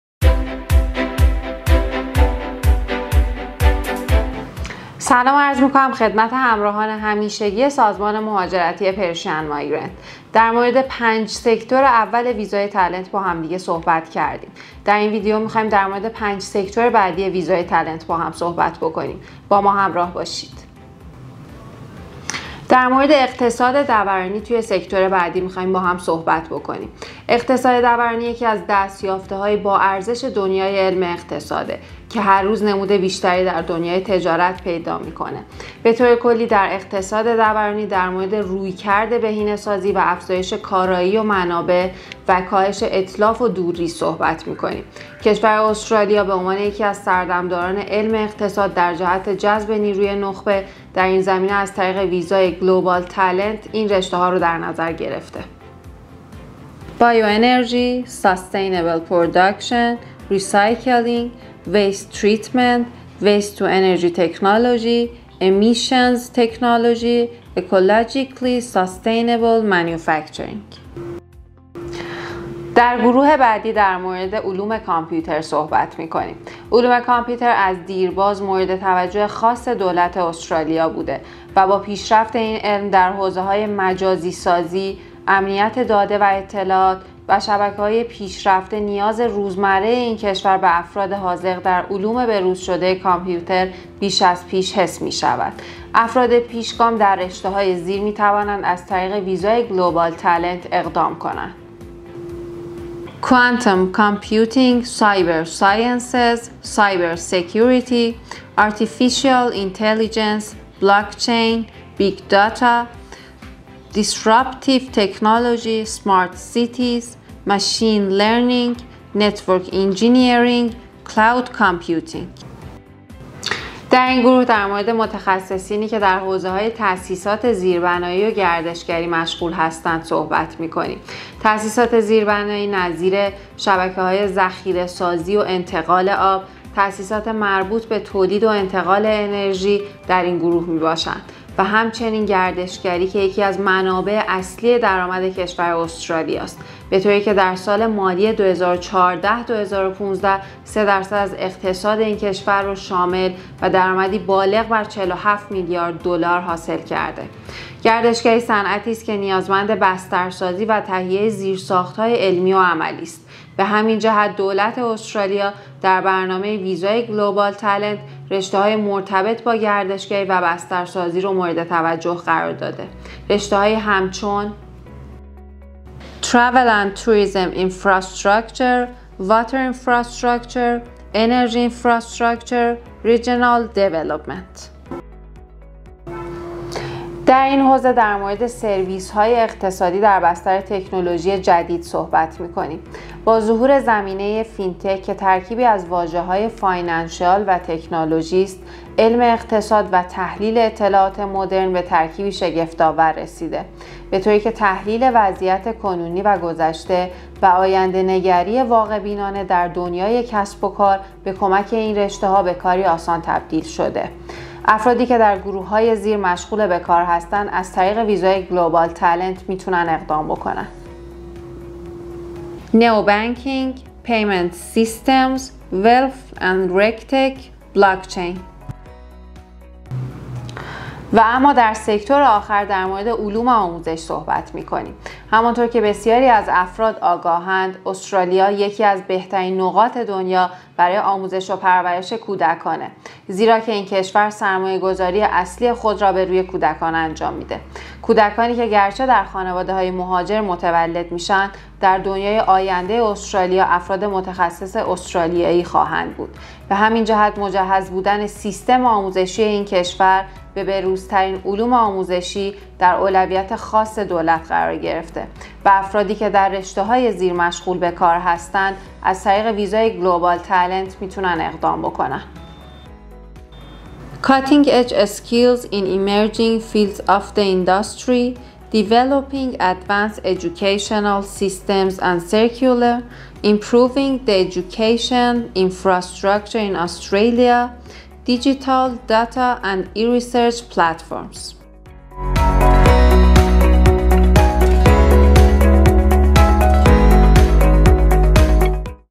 The Australian Talent Visa's Sectors' - Part 2
In this series of videos, we have examined the sections of the talent search visa and the details of the courses of each section. In these videos, you will learn more about each section, subcategories, disciplines, and people who can apply for these sections. In the second part of this series of videos, we have reviewed the final 5 broadcasts of the Talent Visa sectors.